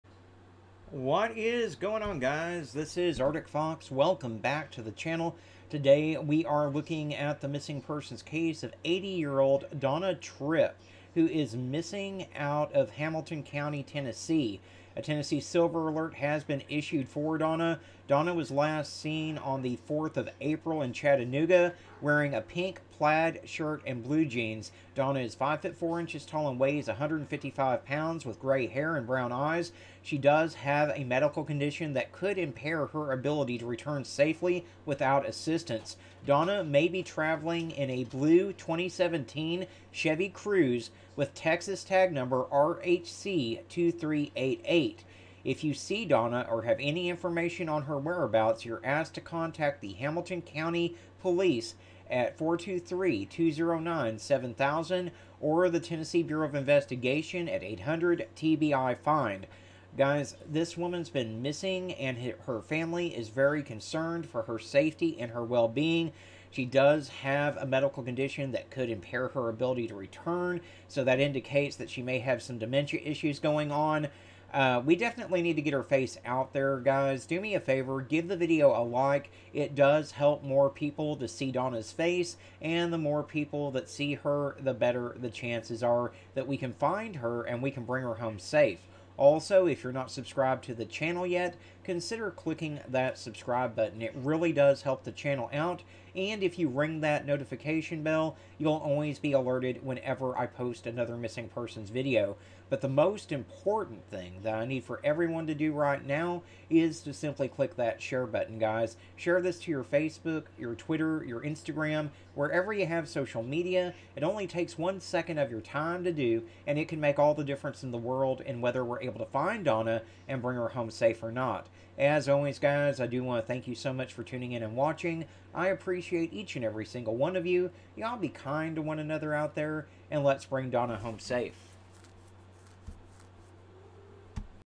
SILVER ALERT ISSUED FOR MISSING TENESSEE WOMAN!!! WHERE IS DONNA TRIPP!!!
ALCOHOLICS ANONYMOUS